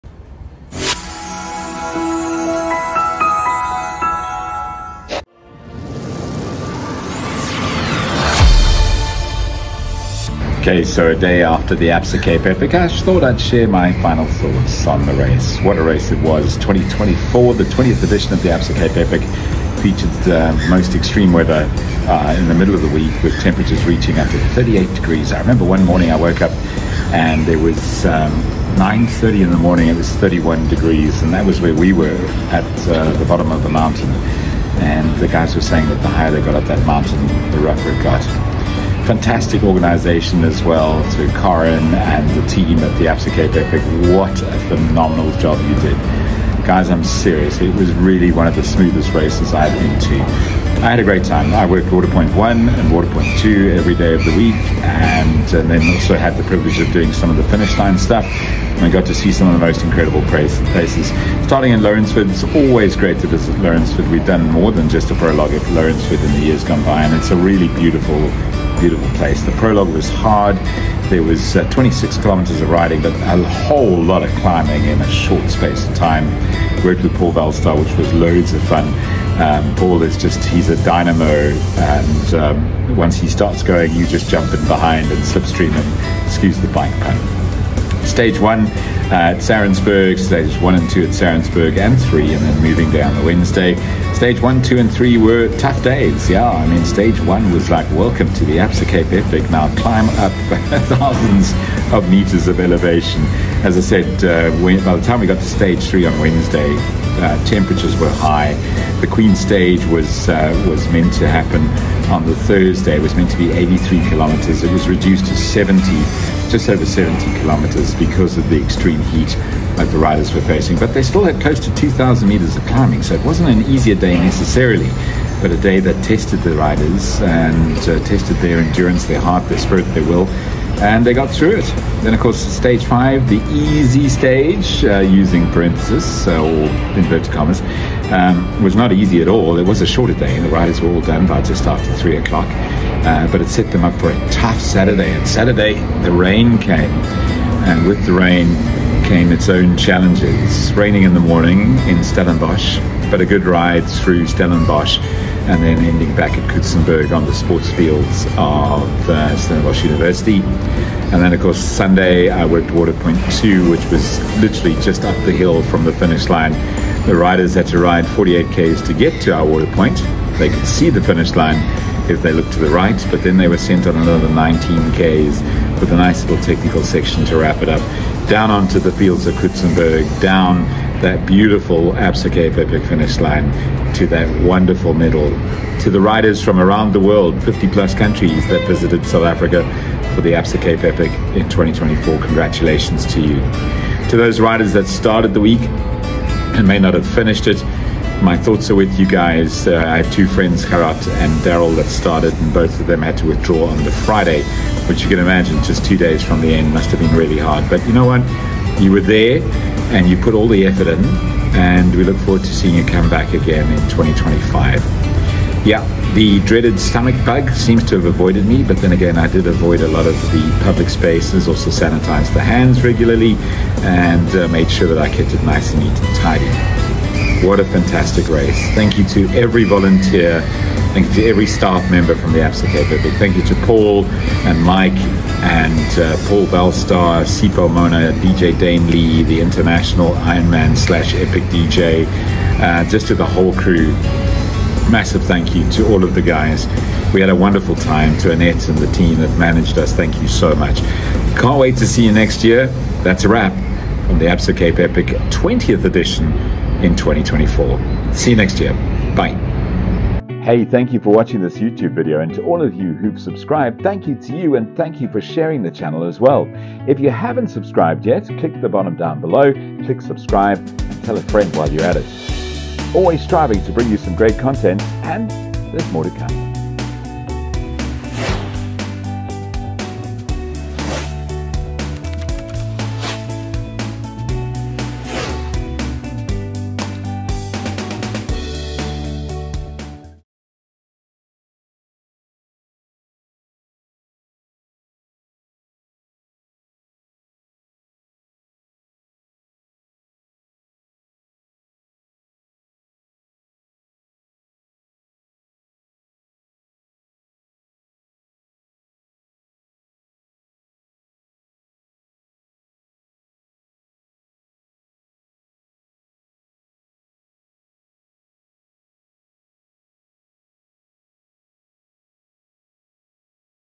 ABSA Cape Epic 2024 Final Thoughts
8 Days came and went so fast that if you blinked you would miss it! Thats a wrap for this year and I wanted to share some of my thoughts of this years race from a Water Point announcers p.o.v. What a year, what a race...